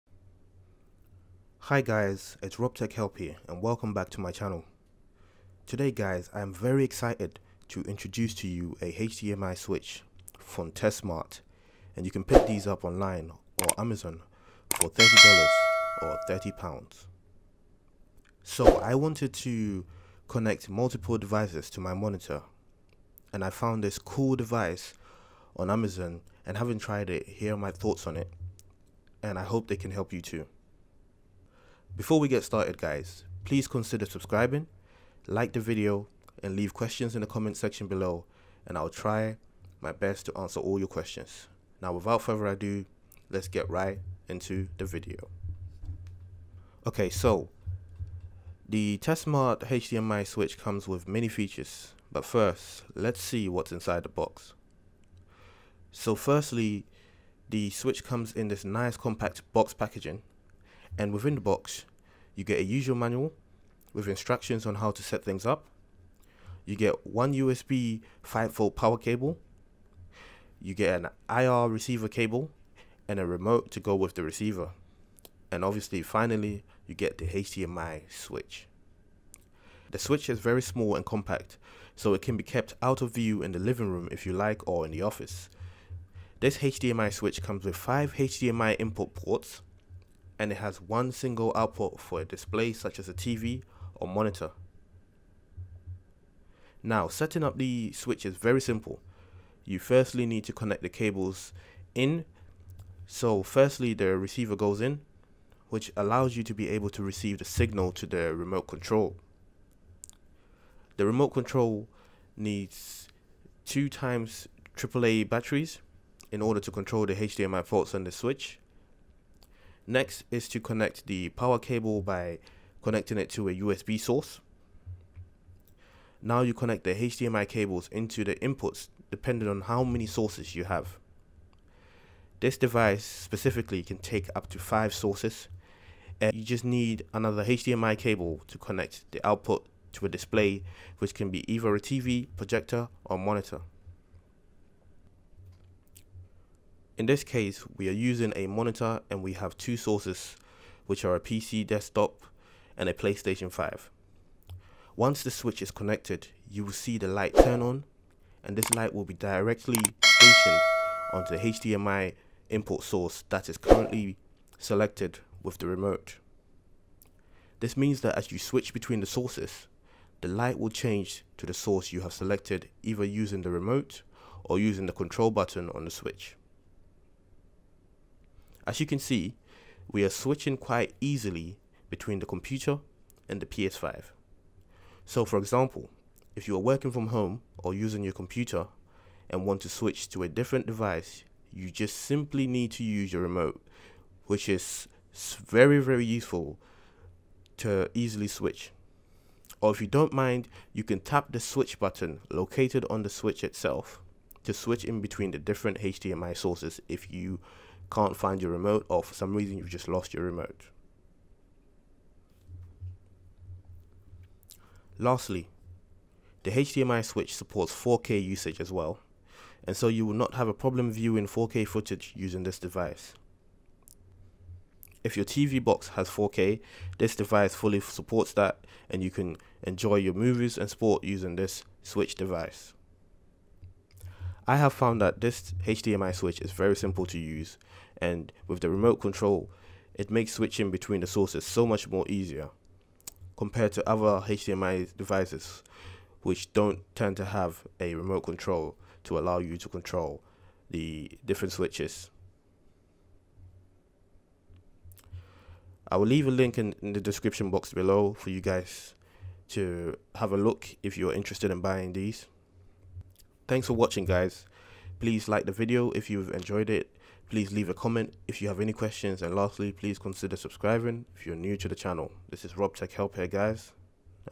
Tesmart HEDMI Switch Review. Connect PC,PS5, Nintendo Switch, XBOX to TV/Monitor At The Same TIme.
Today's review is for this TESmart HDMI switch. Its a device that I use to connect multiple HDMI sources to my TV and monitor.